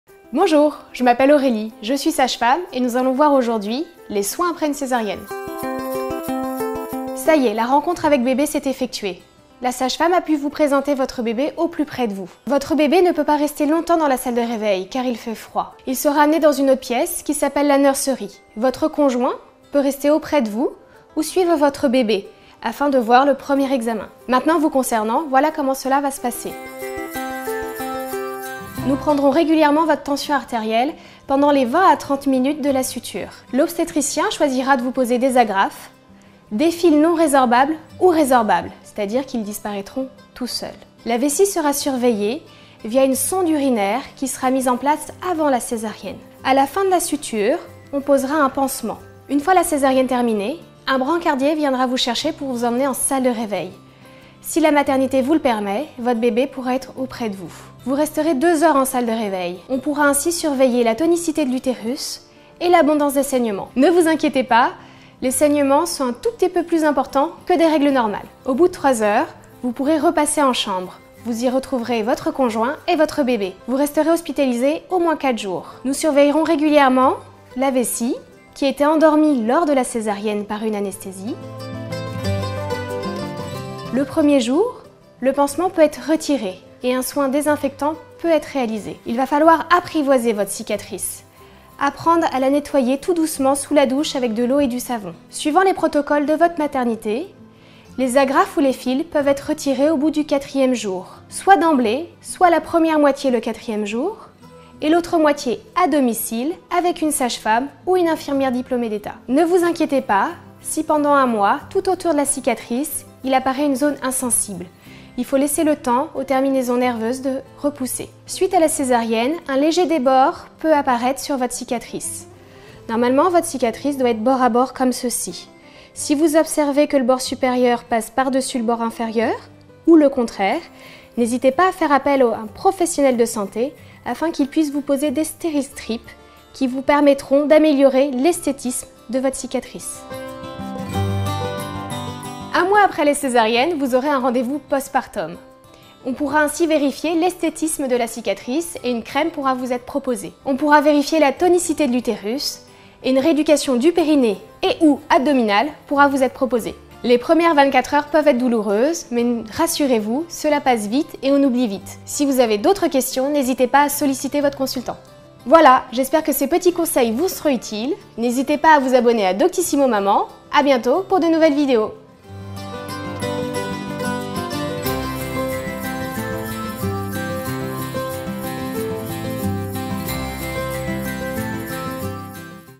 Les soins après une césarienne - Sage femme
Aurélie Boulier, sage-femme, vous explique quels sont les  soins pratiqués après une césarienne ainsi que le suivi mis en place après le retour à la maison.

Production : LED